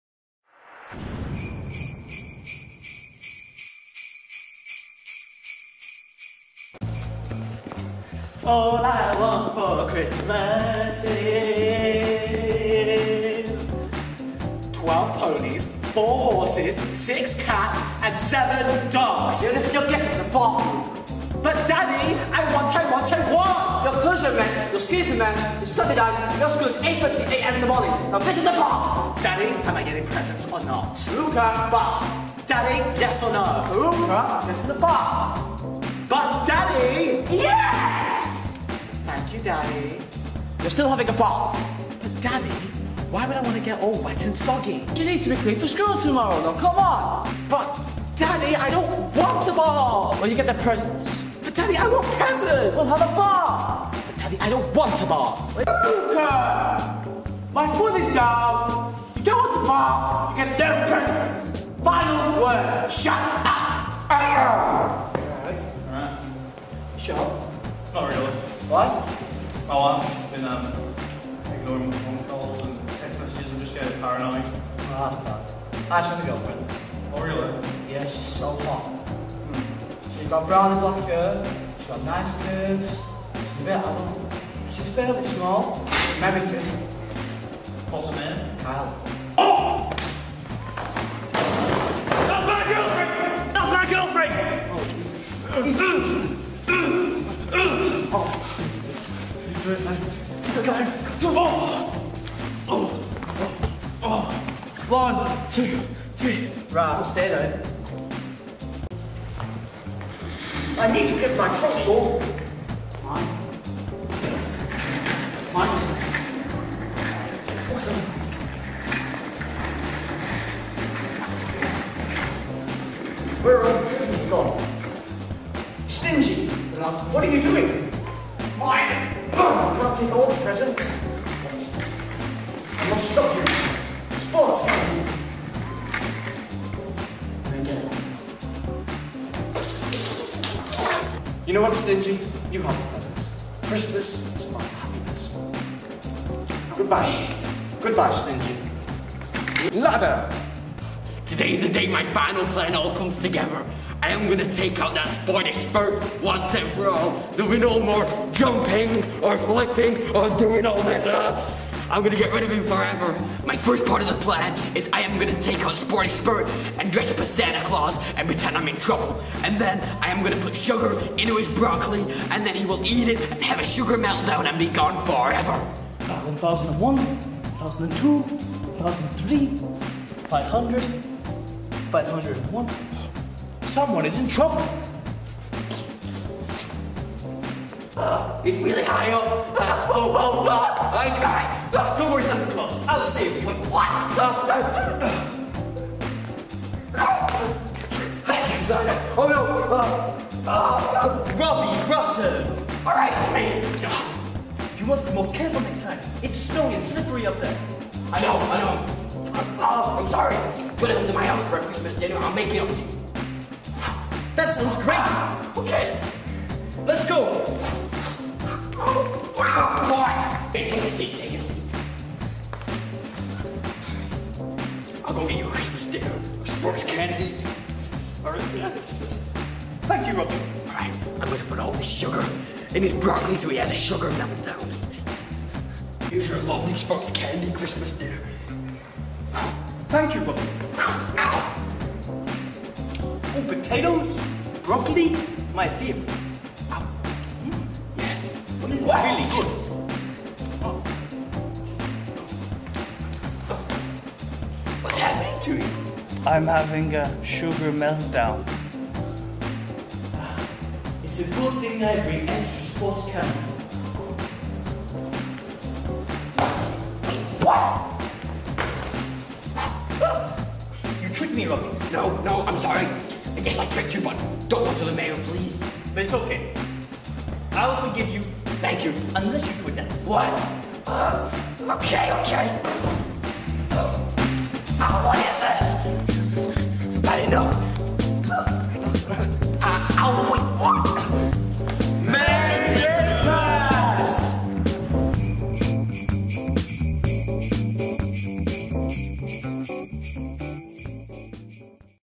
WSA Dance & Drama 'The Level 2 Christmas Special'
The Level 2 students perform a series of short Christmas themed comedy sketches created using improvisation cards.  Each card provided characters involved in a status battle as a result of a Christmas event that had gone wrong.  The work is part of preparations for their Devising Plays Unit.  Merry Christmas!